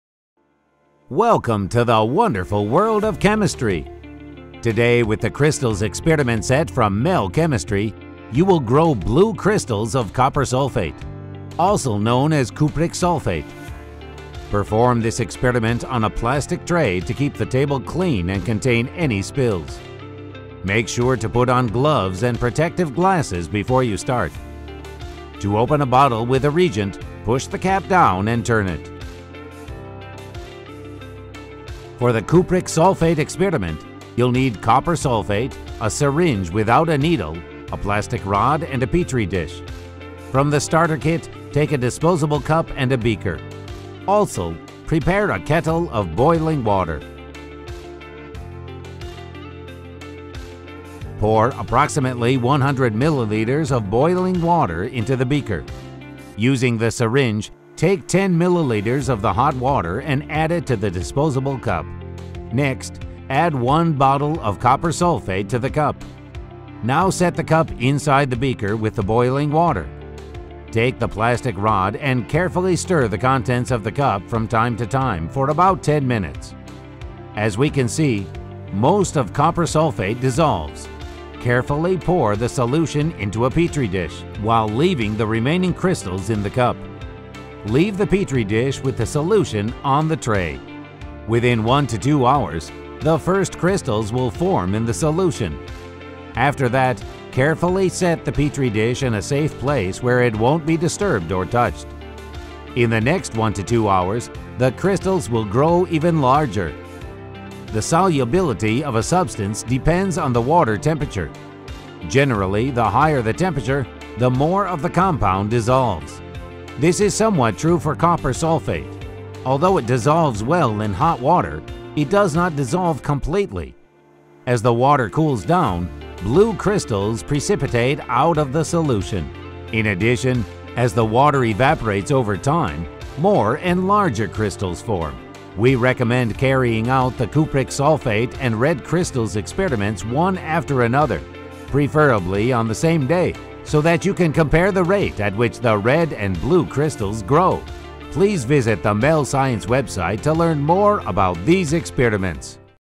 “Cupric sulfate“ from the “Crystals“ set
Safety
• Put on protective gloves and eyewear.
• Conduct the experiment on the plastic tray.
• Observe safety precautions when working with boiling water.

Disposal
Dispose of solid waste together with household garbage.

Step-by-step instructions
1. Pour 100 mL of freshly boiled water in the beaker.
2. Collect 10 mL of water with a syringe.
3. Pour water taken with the syringe into a plastic cup. Add one vial оf copper sulfate CuSO₄ (6.5 g).
4. Place the cup into the beaker with hot water.
5. Stir the contents in the cup for 10 min.
6. Pour out the obtained solution into a Petri dish, so that crystalline sediment remains in the cup.
7. Within an hour, crystals will start growing in the Petri dish. And in a couple of days, the crystals will become even larger!